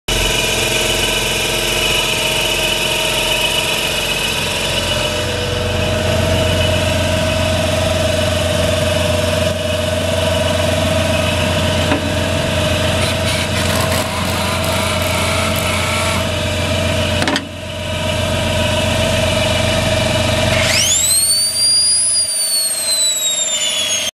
Hydraulic run Air Compressor on 2008 Dodge Service Rig Selling on Big Iron Online Auction May 24, 2017